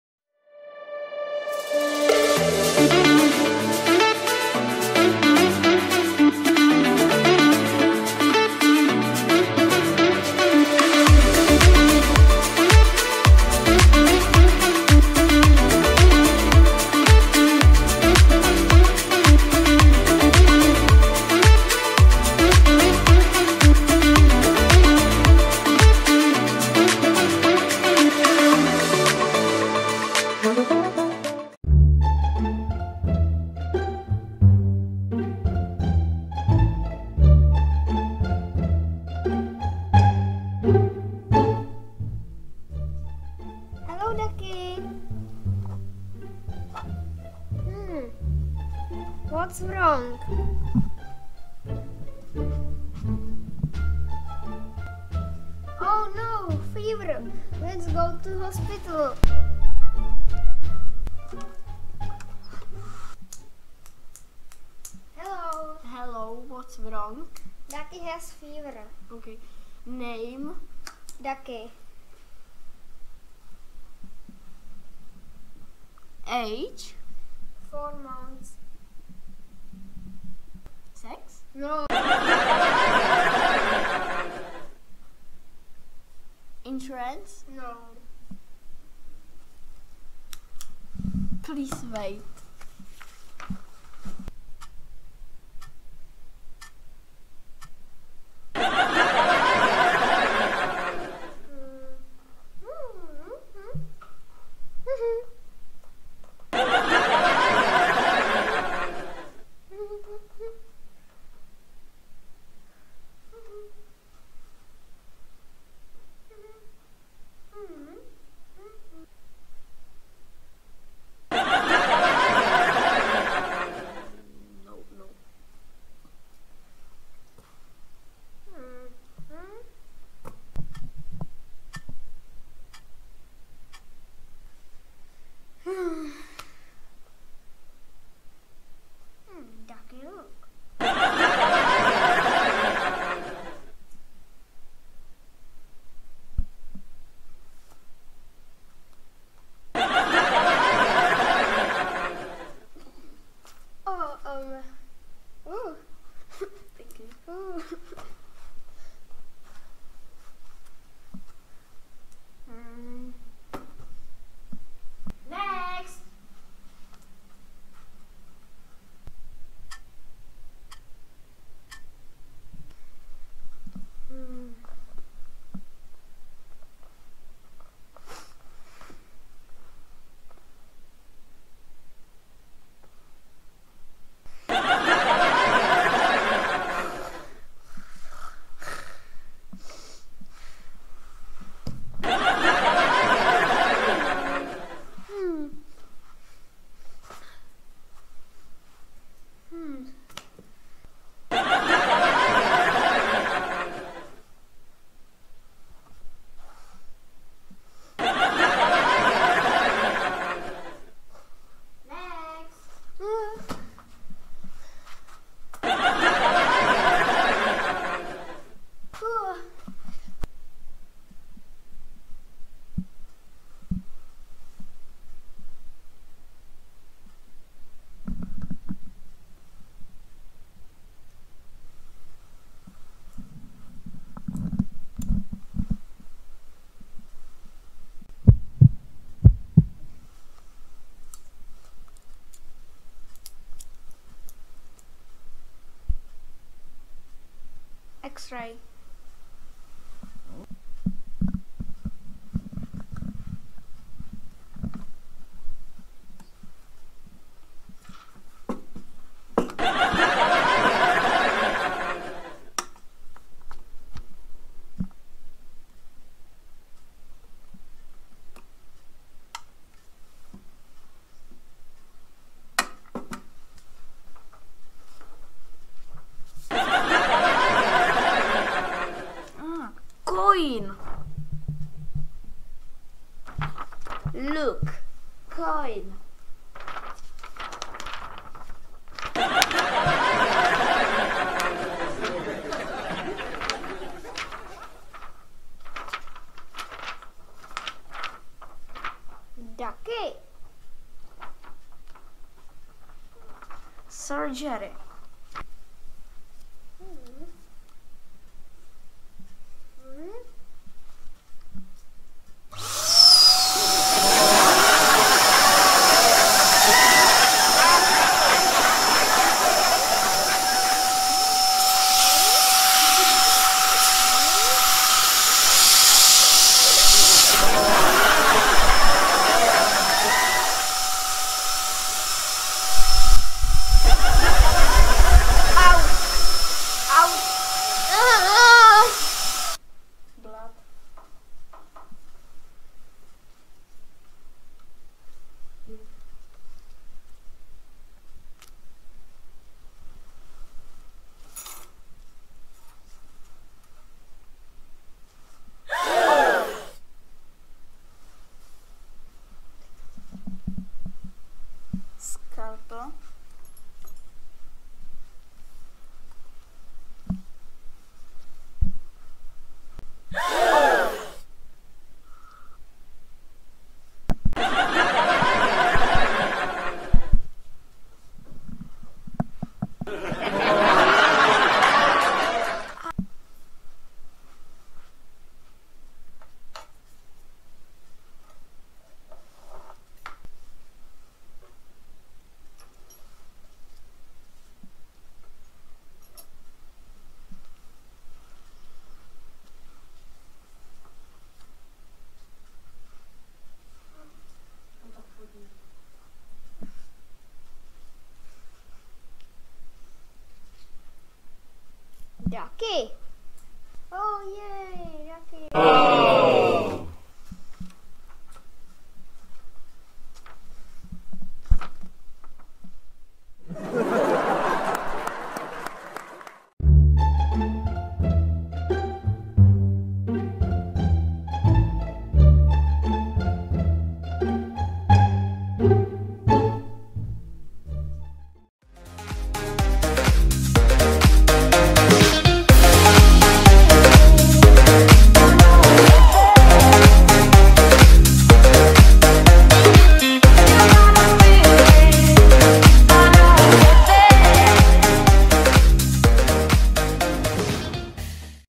Minion and Ducky - Ep. 1 - Ducky needs surgery 🥼🩺💉🩸 comedy series
S01E01 Ducky needs surgery

Ducky má horečku a tak se Minion rozhodne vzít ho do nemocnice ...

Ducky has a fever, so Minion decides to take him to the hospital ...